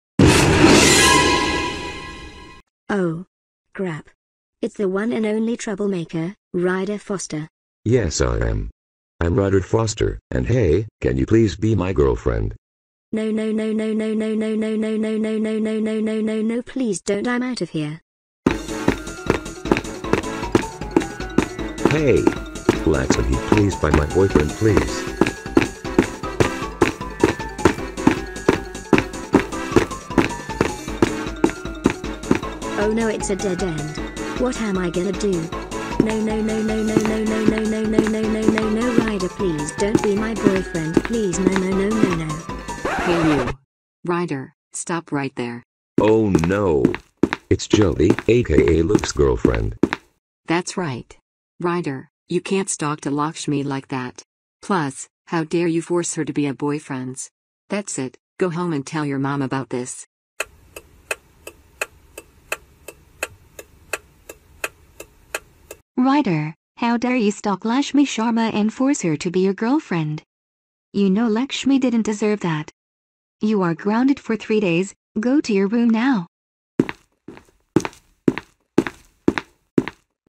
Ryder Foster Tries To Stalk Lakshmi/Grounded (Short)
Another short video today bc why not. Credit to @StarJesusTheLatinAmericanBoi05 for Ryder and his mom.

Programs used:
Bandicam
Wrapper Offline

My Discord Tag: FilbertTheVyonderBoy2K4#8811